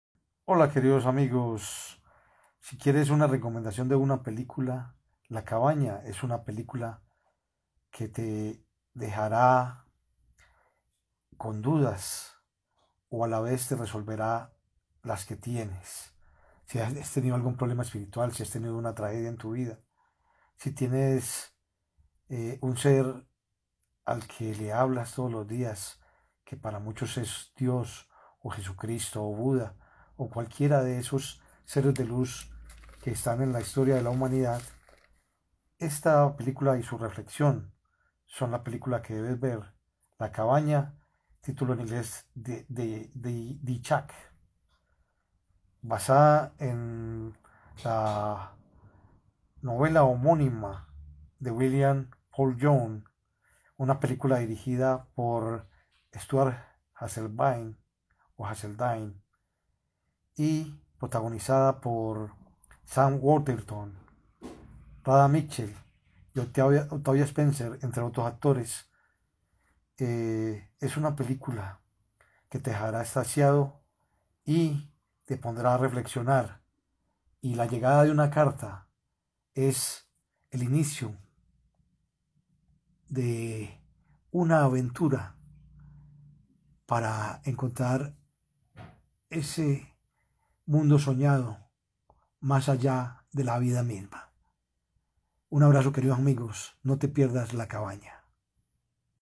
"LA CABAÑA" Netflix recomendada.
"La Cabaña". Es una película basada en el libro del mismo nombre del escritor William Paul Young, en ingles :"The Shack".
Es un film de 2017, dirigido por Stuart Hazeldine. Con actores como Sam Worthington, Radha Mitchell. Octavia Spencer, Megan Charpentier, Gage Monroe, , Amelie Eve, Avraham Aviv Alush, Sumire, Tim McGraw, Alice Braga, Graham Greene, Ryan Robbins, Carson Reaume.